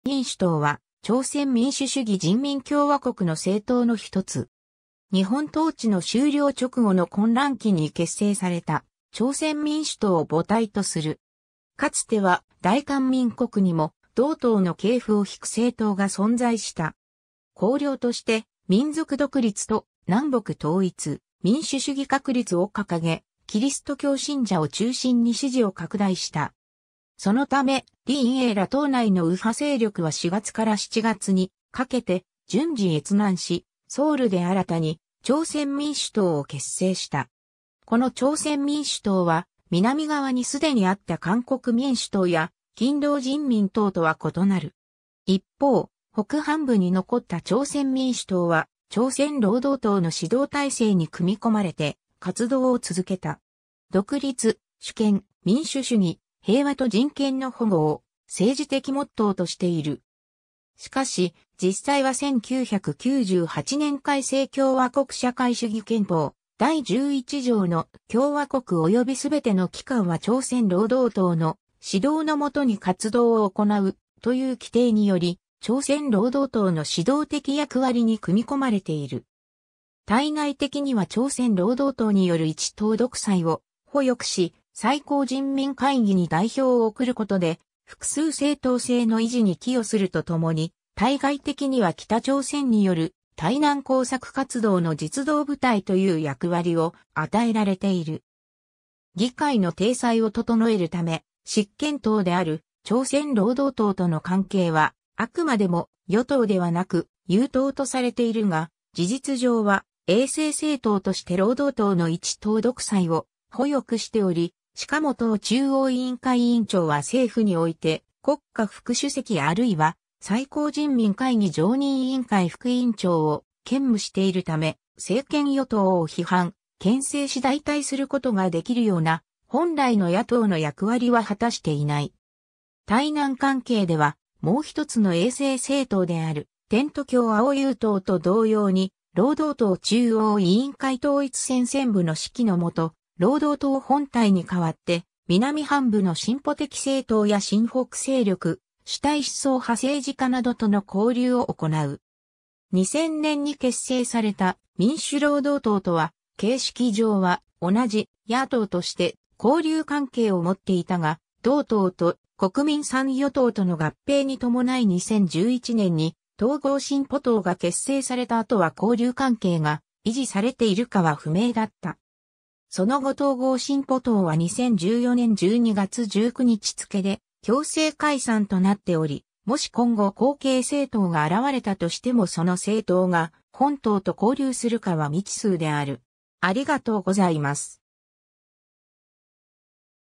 朝鮮社会民主党
朝鮮・韓国の社会主義
朝鮮民主党の結成大会（1945年11月3日） 朝鮮社会民主党（ちょうせんしゃかいみんしゅとう、조선사회민주당）は、朝鮮民主主義人民共和国の政党の一つ。
日本統治の終了直後の混乱期に結成された朝鮮民主党（ちょうせんみんしゅとう、조선민주당）を母体とする。
かつては、大韓民国にも同党の系譜を引く政党が存在した。
 綱領として「民族独立」と「南北統一」、「民主主義確立」を掲げ、キリスト教信者を中心に支持を拡大した。
 そのため、李允栄ら党内の右派勢力は4月から7月にかけて順次越南し、ソウルで新たに朝鮮民主党を結成した。
この朝鮮民主党は、南側に既にあった韓国民主党（韓民党）や勤労人民党とは異なる。
 一方、北半部に残った朝鮮民主党は、朝鮮労働党の指導体制に組み込まれて活動を続けた。
 「独立、主権、民主主義、平和と人権の保護」を政治的モットーとしている。
しかし、実際は1998年改正共和国社会主義憲法第11条の「共和国およびすべての機関は朝鮮労働党の指導のもとに活動を行う」という規定により、朝鮮労働党の指導的役割に組み込まれている。
対内的には朝鮮労働党による一党独裁を輔翼し、最高人民会議に代表を送ることで複数政党制の維持に寄与すると共に、対外的には北朝鮮による対南工作活動の実働部隊という役割を与えられている。
 議会の体裁を整えるため、執権党である朝鮮労働党との関係は、あくまでも与党ではなく「友党」とされているが、事実上は衛星政党として労働党の一党独裁を輔翼しており、しかも党中央委員会委員長は政府において国家副主席（1998年以前）あるいは最高人民会議常任委員会副委員長を兼務しているため、政権与党を批判・牽制し代替することができるような、本来の野党の役割は果たしていない。
 対南関係では、もう一つの衛星政党である天道教青友党と同様に、労働党中央委員会統一戦線部の指揮の下、労働党本体に代わって南半部（大韓民国）の進歩的政党や親北勢力、主体思想派政治家などとの交流を行う。
2000年に結成された民主労働党とは、形式上は同じ「野党」として交流関係を持っていたが、同党と国民参与党との合併に伴い2011年に統合進歩党が結成された後は交流関係が維持されているかは不明だった。
その後統合進歩党は2014年12月19日付で強制解散となっており、もし今後後継政党が現れたとしてもその政党が本党と交流するかは未知数である。